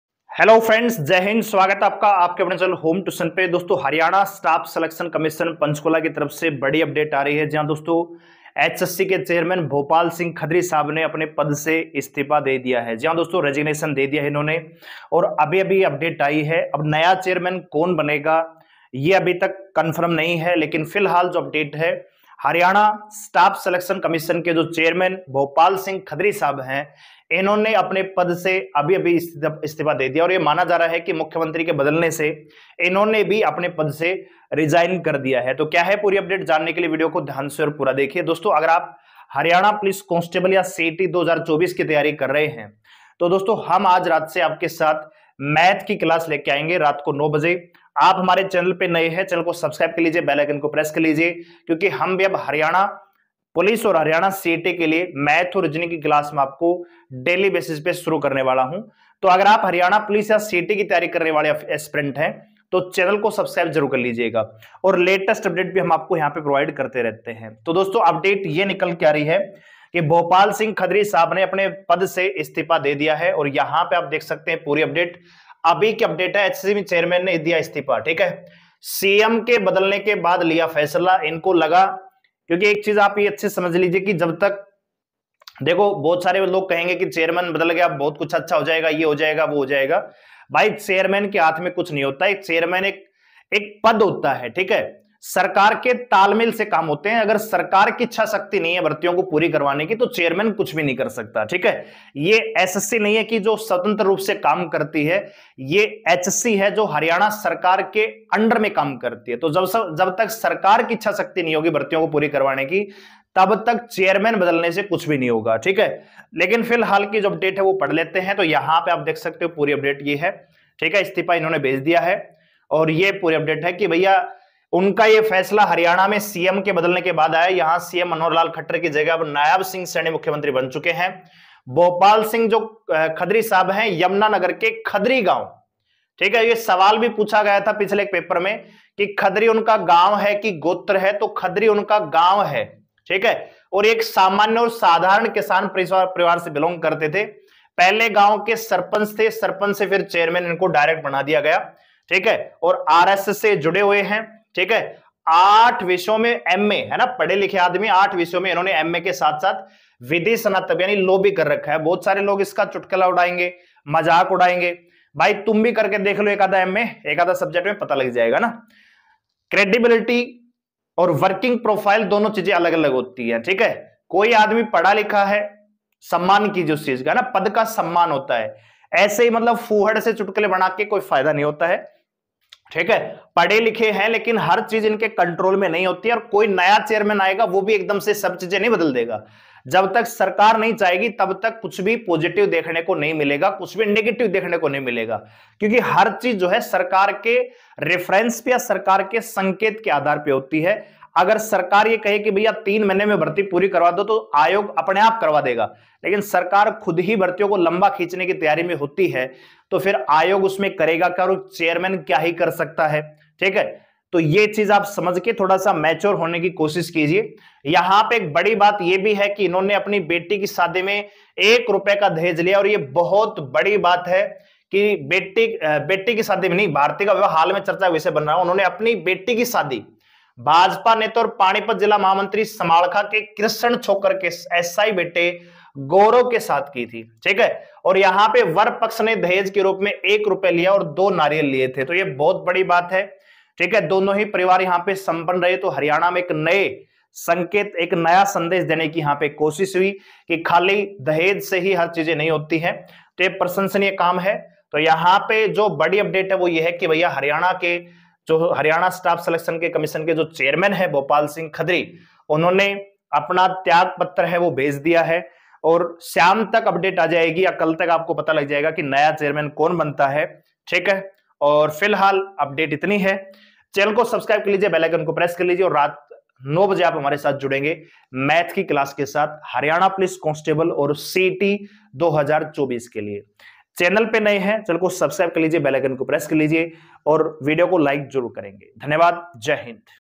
HSSC Breaking 🔥 | भोपाल सिंह खदरी ने चेयरमैन पद से दिया इस्तीफा | जल्दी देखिए जी
HSSC Breaking 🔥 | भोपाल सिंह खदरी ने चेयरमैन पद से दिया इस्तीफा | जल्दी देखिए जी | HSSC Breaking 🔥🔥 | HSSC Chairman Resign

आपका अपना चैनल :- HOME TUITION